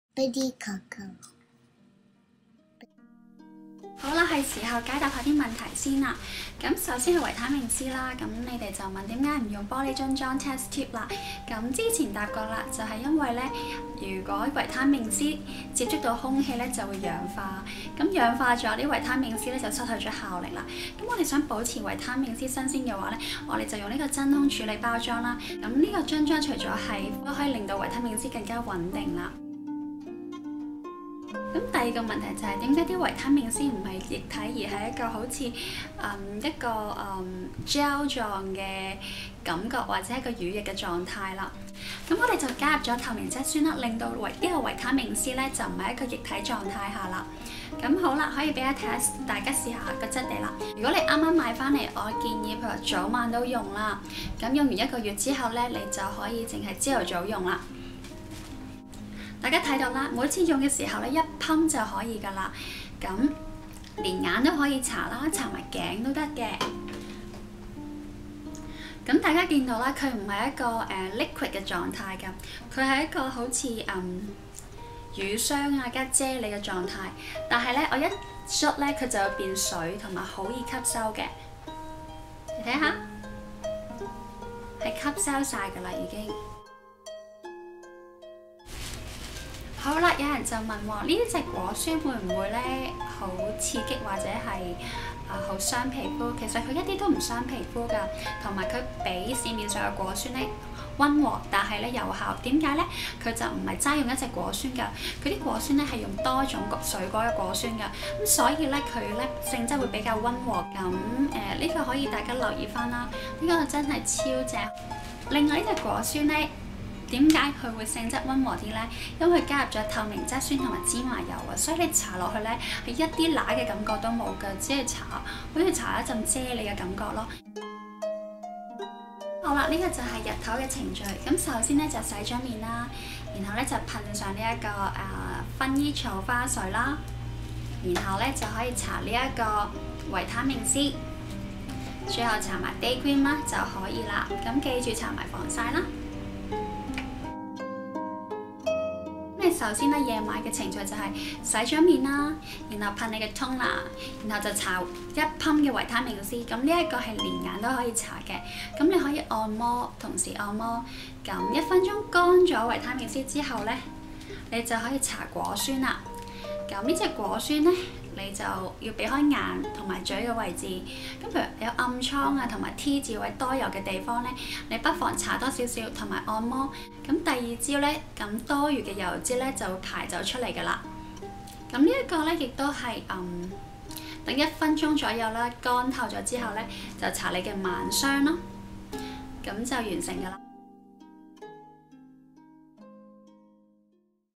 Beautykoko產品Q&A Day and Night Skincare routine
Hi everyone,
Thanks for your questions for about Hylunia's products! Please let me know if you have more questions about our products.